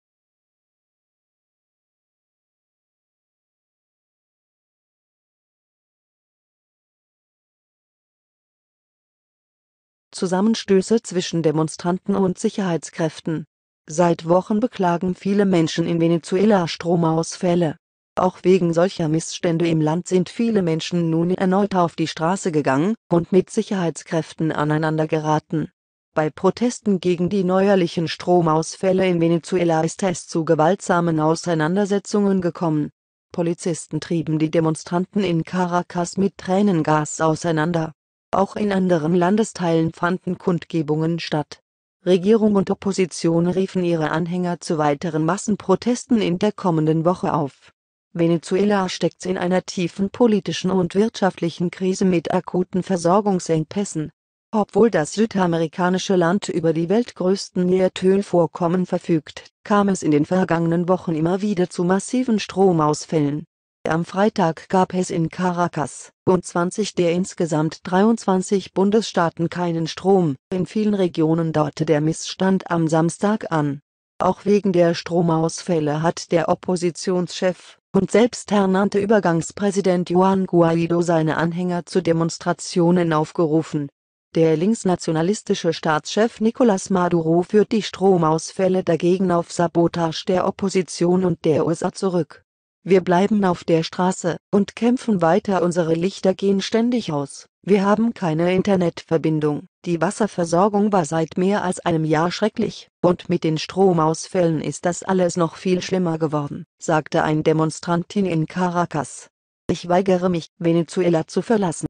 Zusammenstöße zwischen Demonstranten und Sicherheitskräften
Seit Wochen beklagen viele Menschen in Venezuela Stromausfälle. Auch wegen solcher Missstände im Land sind viele Menschen nun erneut auf die Straße gegangen - und mit Sicherheitskräften aneinander geraten.

Bei Protesten gegen die neuerlichen Stromausfälle in Venezuela ist es zu gewaltsamen Auseinandersetzungen gekommen. Polizisten trieben die Demonstranten in Caracas mit Tränengas auseinander. Auch in anderen Landesteilen fanden Kundgebungen statt. Regierung und Opposition riefen ihre Anhänger zu weiteren Massenprotesten in der kommenden Woche auf.

Venezuela steckt in einer tiefen politischen und wirtschaftlichen Krise mit akuten Versorgungsengpässen. Obwohl das südamerikanische Land über die weltgrößten Erdölvorkommen verfügt, kam es in den vergangenen Wochen immer wieder zu massiven Stromausfällen. Am Freitag gab es in Caracas und 20 der insgesamt 23 Bundesstaaten keinen Strom, in vielen Regionen dauerte der Missstand am Samstag an.

Auch wegen der Stromausfälle hat der Oppositionschef und selbsternannte Übergangspräsident Juan Guaidó seine Anhänger zu Demonstrationen aufgerufen. Der linksnationalistische Staatschef Nicolás Maduro führt die Stromausfälle dagegen auf "Sabotage" der Opposition und der USA zurück.

"Wir bleiben auf der Straße und kämpfen weiter"

"Unsere Lichter gehen ständig aus, wir haben keine Internetverbindung, die Wasserversorgung war seit mehr als einem Jahr schrecklich, und mit den Stromausfällen ist das alles noch viel schlimmer geworden", sagte ein Demonstrantin in Caracas.

"Ich weigere mich, Venezuela zu verlassen, weil ich sicher bin, dass es hier so vieles gibt, für das es sich zu kämpfen lohnt", sagte eine andere Demonstrantin. "Wir bleiben auf der Straße und kämpfen weiter."

Guaidó rief zu neuen Massenprotesten gegen die Regierung am kommenden Samstag auf. Auch Maduro appellierte an seine Unterstützer, auf die Straße zu gehen und "Nein zum terroristischen US-Imperialismus zu sagen". Die "colectivos" - teilweise bewaffnete Gruppen von Maduro-Unterstützern - sollten gegenüber gewaltsamen Protesten "null Toleranz" zeigen.

Die USA haben Guaidó ebenso wie Deutschland und rund 50 weitere Länder als Übergangspräsidenten anerkannt. Russland zählt dagegen zu den wichtigsten Unterstützern von Maduro.